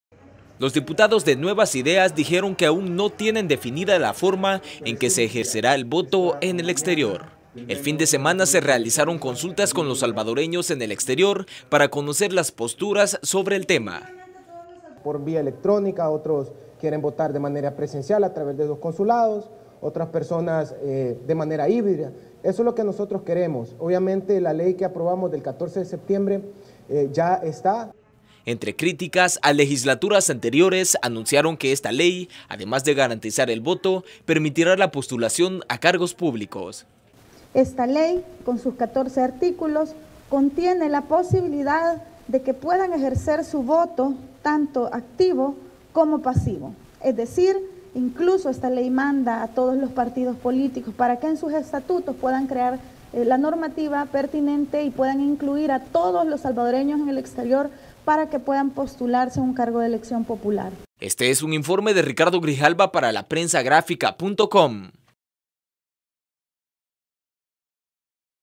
Asamblea aún no define la forma del voto en el exterior
Los diputados de Nuevas Ideas dijeron que aún no tienen definida la forma en que se ejercerá el voto en el exterior.

¡Visítenos!